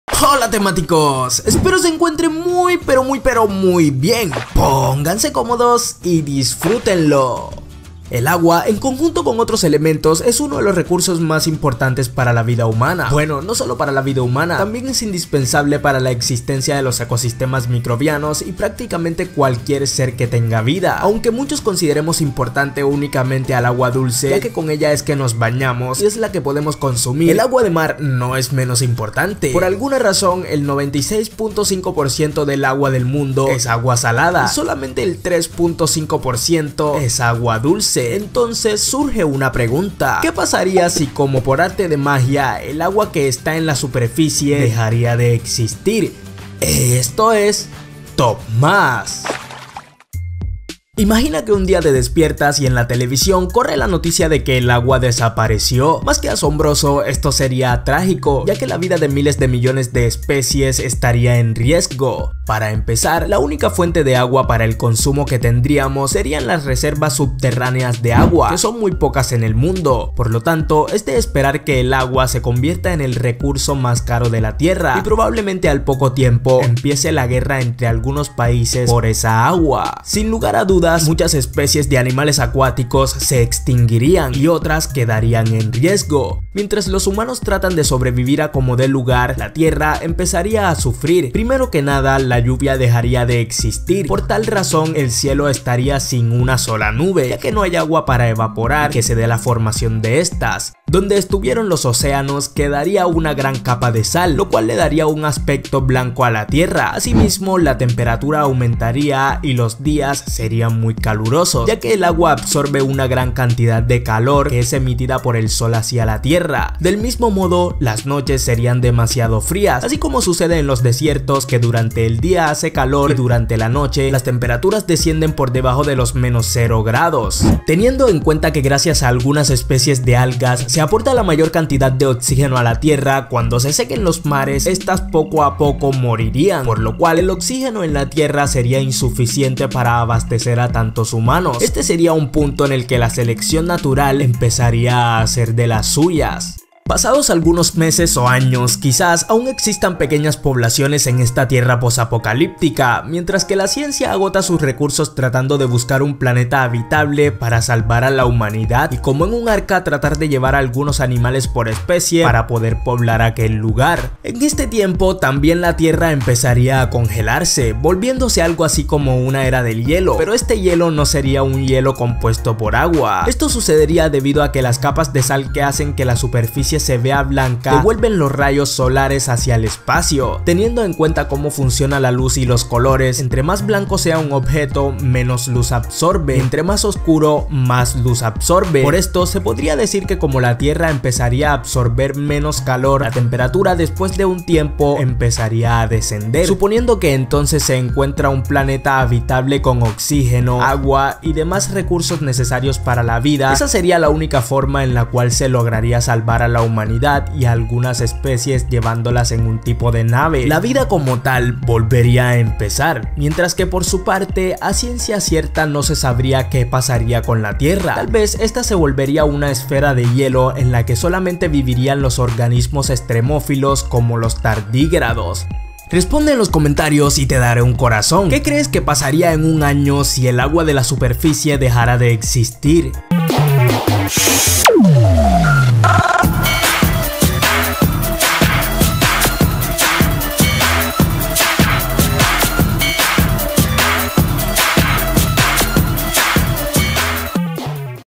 ¿Que pasaría si el océano desaparece MAGICAMENTE?
Mira lo que pasaría si el mundo se quedara sin agua de la noche a la mañana, realmente es sorprendente la cadena de resultados y consecuencias que esto traería para la humanida, sólo Dios y la ciencia podrían salvar a la humanidad, suscribete, comenta, comparte y dale me gusta